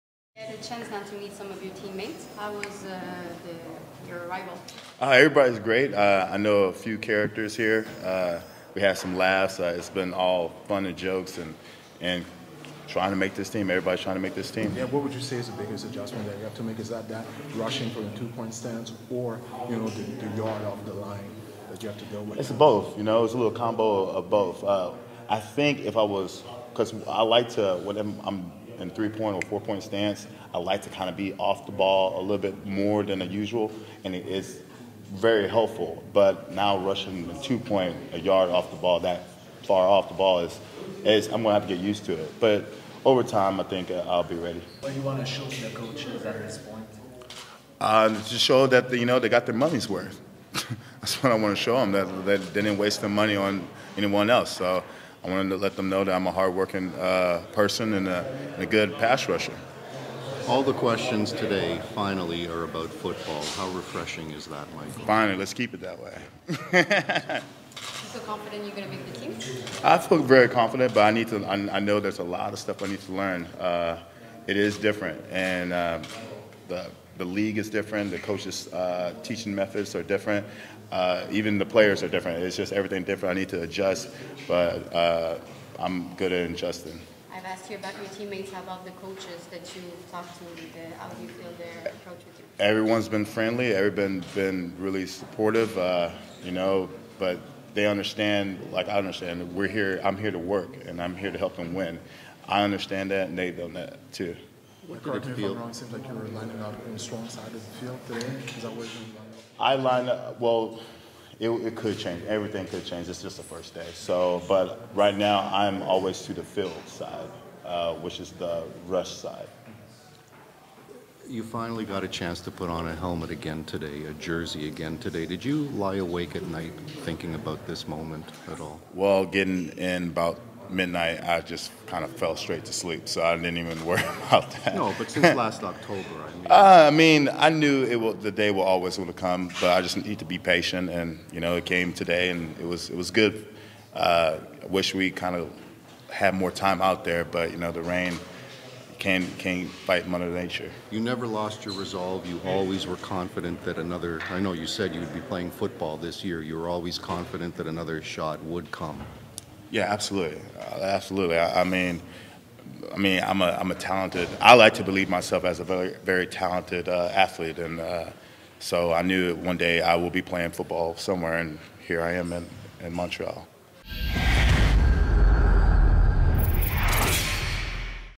Michael Sam's First CFL Practice
Michael Sam attends his first practice with the Canadian Football League's Montreal Alouettes. He was thrilled to be back on the field and knows he has lots to learn about the CFL game.